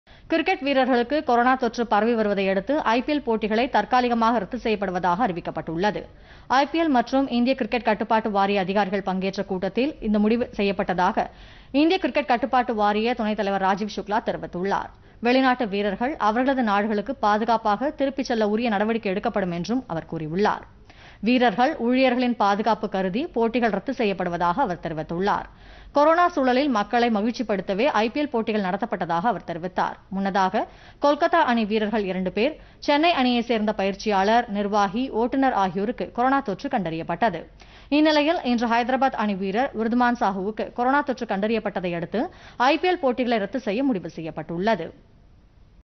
ஐபிஎல் போட்டிகள் தற்காலிகமாக ரத்து
கிரிக்கெட் வீரர்களுக்கு கொரோனா தொற்று பரவல் - ஐபிஎல் போட்டிகளை தற்காலிகமாக ரத்து செய்தது இந்திய கிரிக்கெட் கட்டுப்பாட்டு வாரியம்.